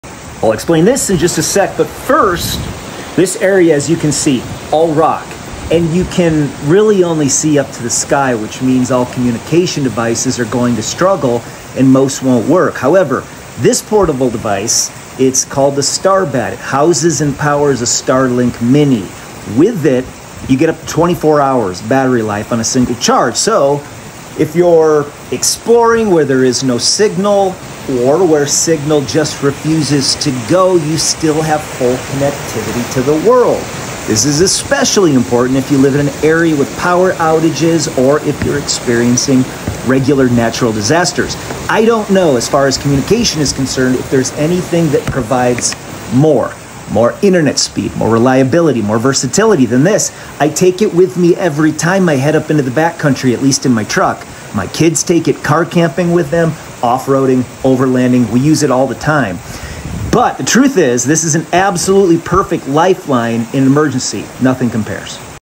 Off-Grid Starlink Power Solution | Star-Batt Mini Overview
Portable power made for real off-grid use.

The Star-Batt Mini Battery keeps your Starlink Mini running up to 24 hours.

Rugged IP65-rated case, LiFePO4 chemistry for long life, and solar or vehicle charging options.

Charge phones, tablets, or laptops while keeping your connection stable anywhere.

Reliable, efficient, and built to last.